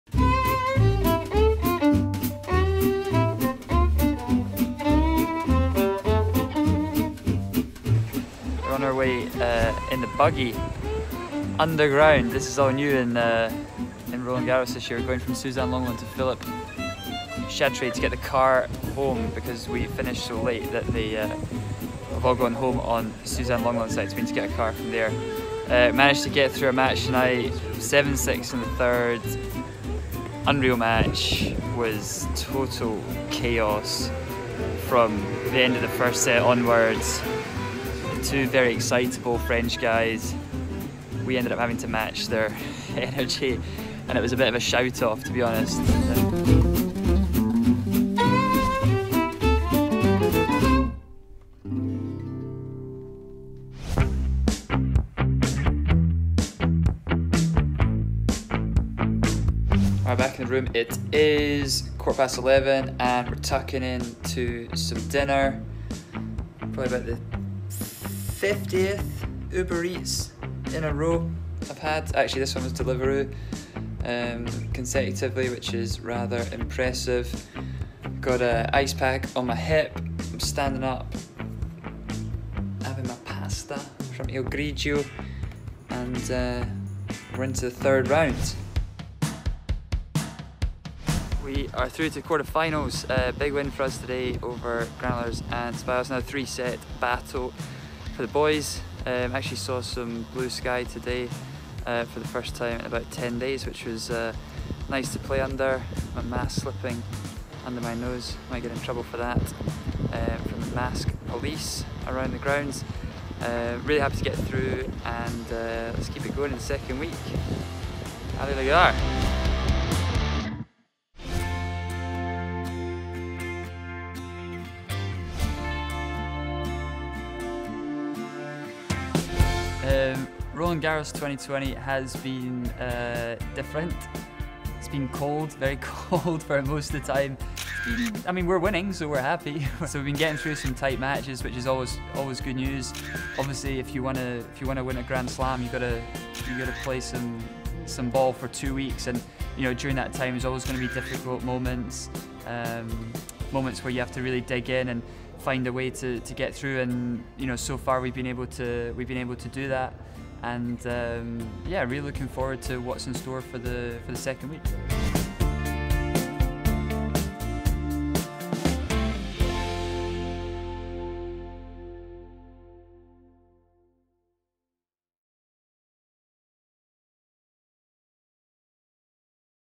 My Tennis Life | Roland Garros 2020 | Jamie Murray
Behind the scenes from Roland Garros 2020, join me in Paris!

For Tennis news, results and updates, visit TENNIS.COM